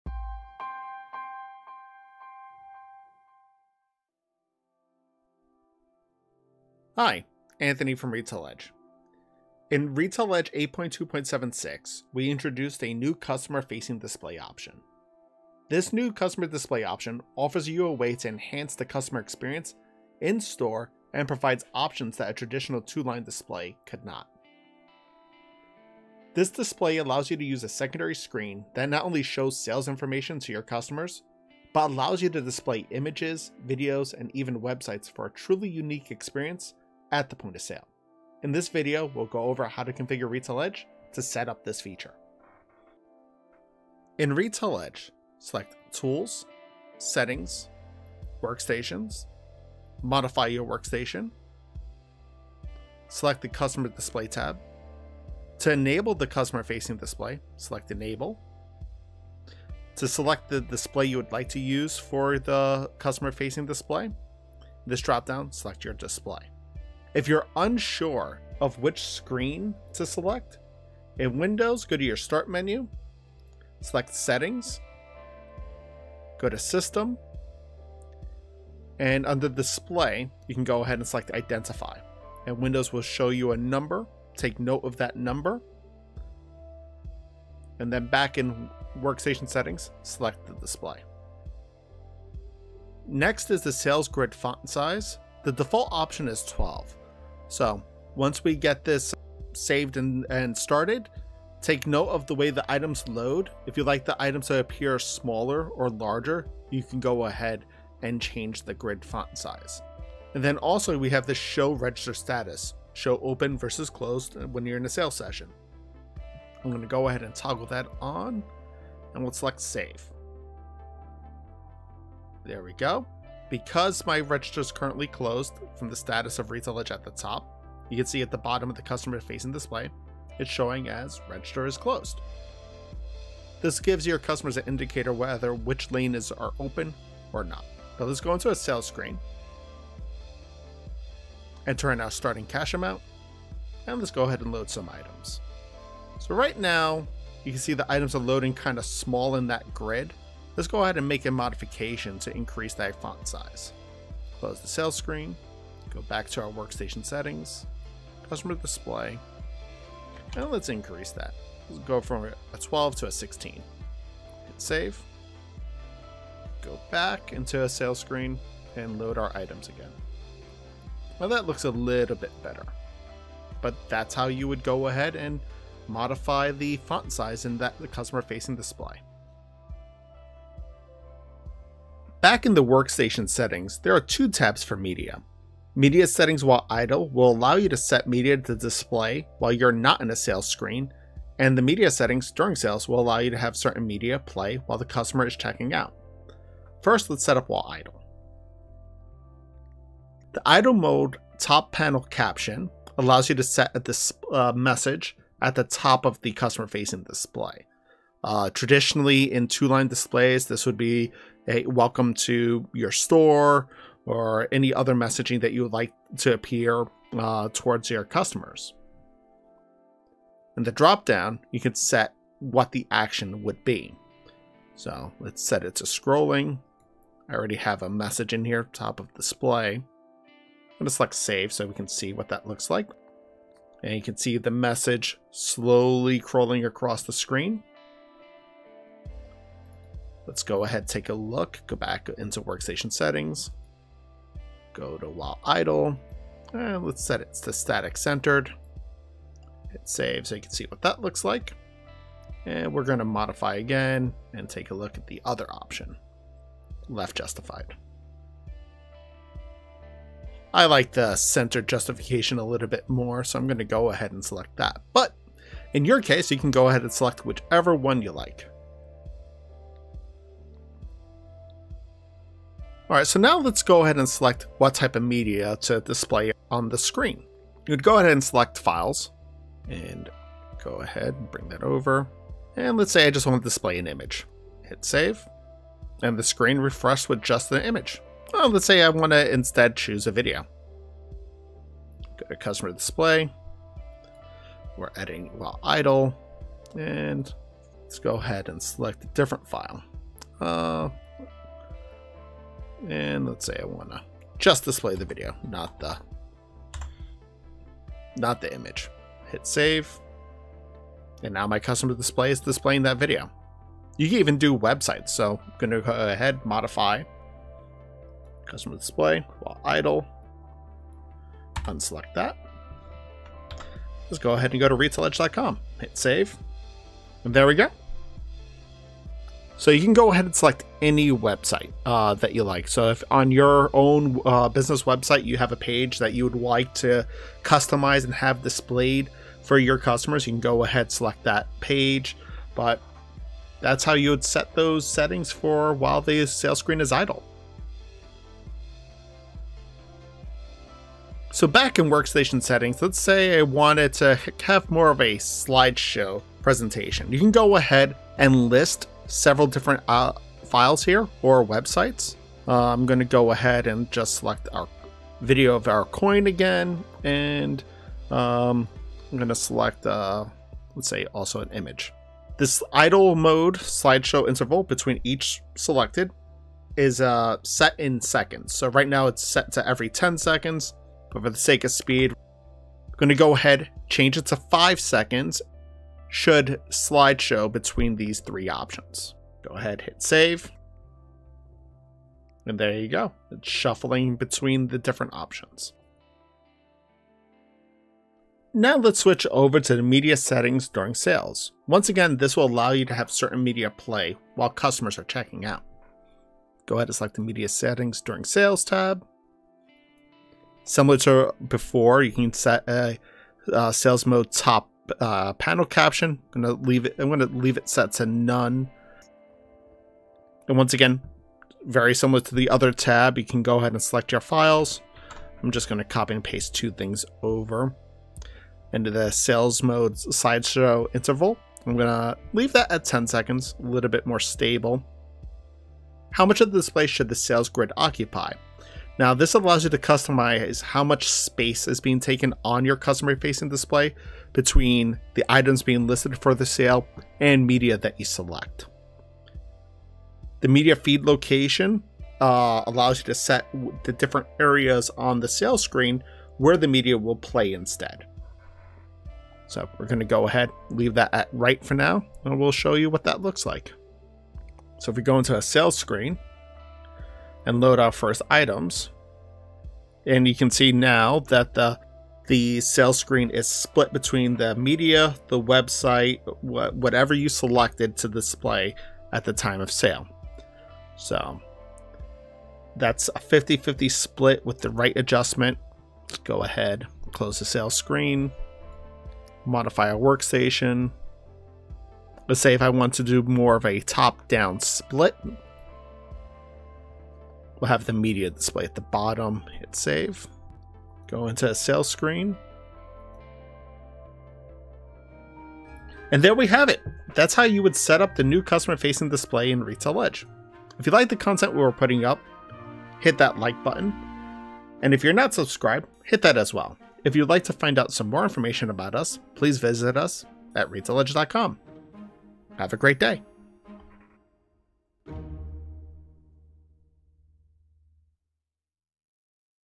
Customer Facing Display Configurations
This new Customer Display option offers you a way to enhance your customer's experience in store and provides options that a traditional two line display could not. This display allows you to use a secondary Screen that not only shows sales information to your customers, but allows you to display images and videos and even websites for a truly unique experience at the point of sale. This video goes over how to configure those settings in RetailEdge.